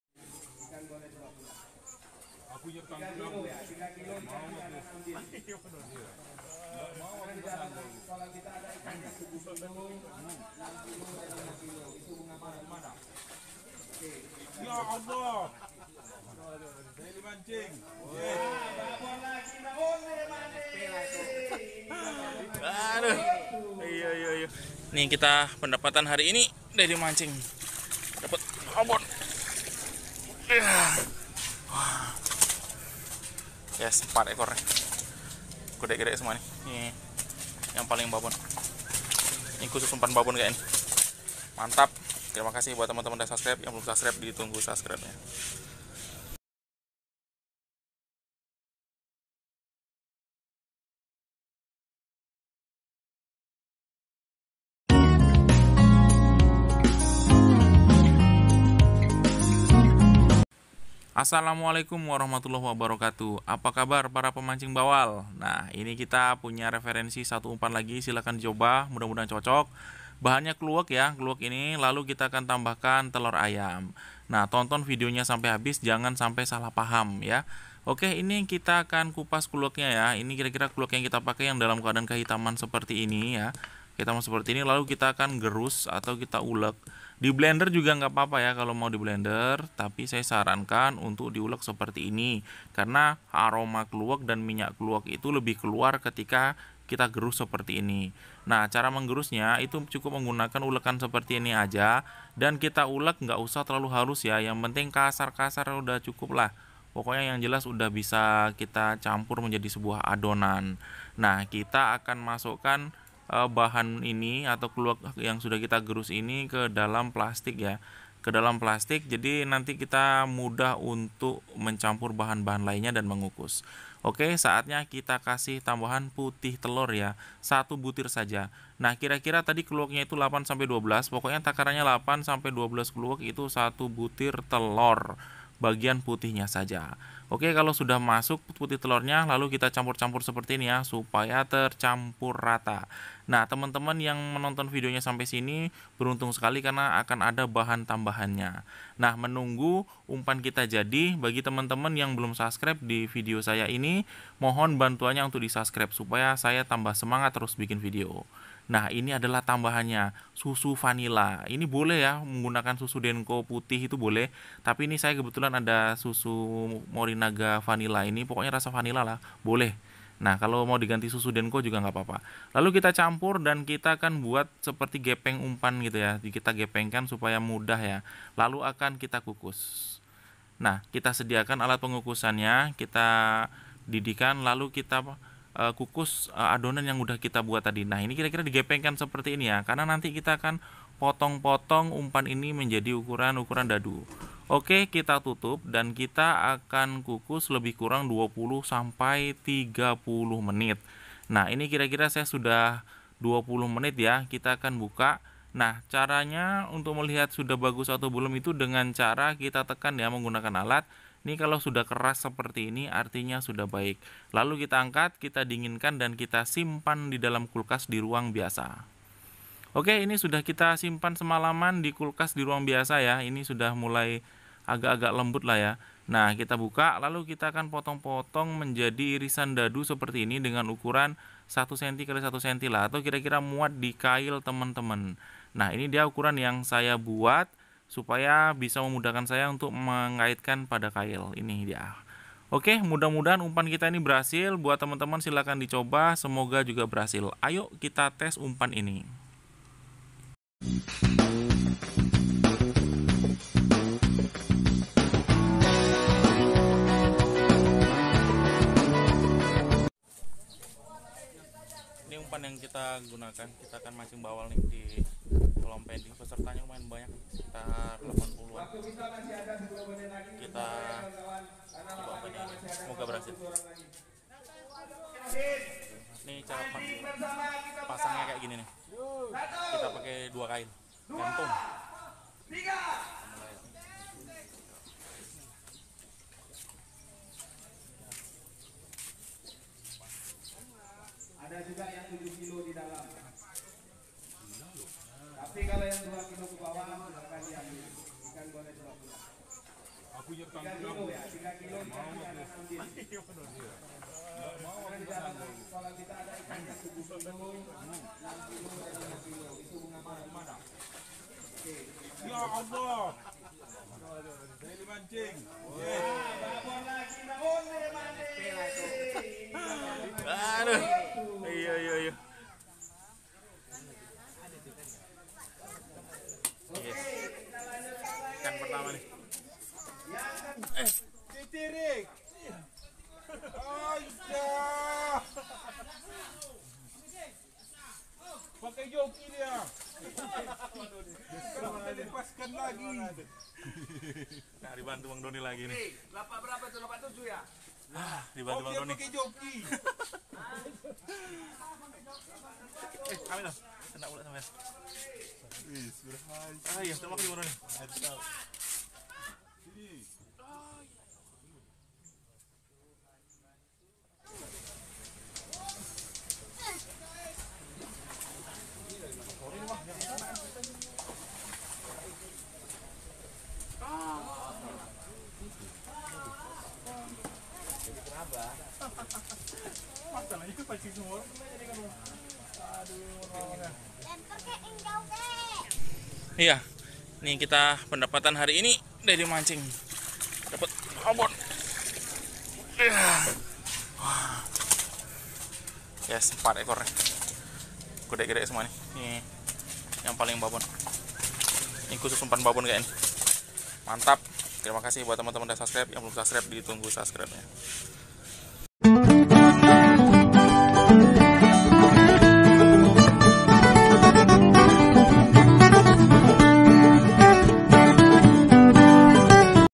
TERBUKTI AMPUH | Umpan Bawal Paling Jitu Media Kluwek || Umpan Bawal Harian Kluwek Kukus
bahan :
1. kluwek 8-12 butir
2. telur ayam 1 butir
3. susu bubuk vanila 2 sdm
kukus kluwek 20 menit. dinginkan dan potong dadu.